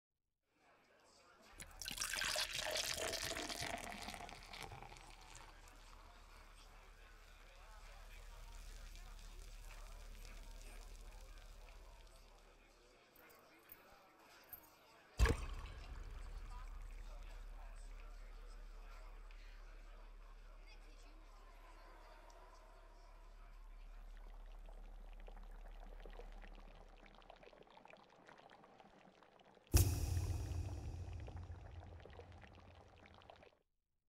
Øl
Føroysk sjónvarpslýsing frá 90unum um at ikki drekka rúsdrekka og koyra samstundis. Lýsingin varð gjørd fyri Ráðið fyri Ferðslutrygd.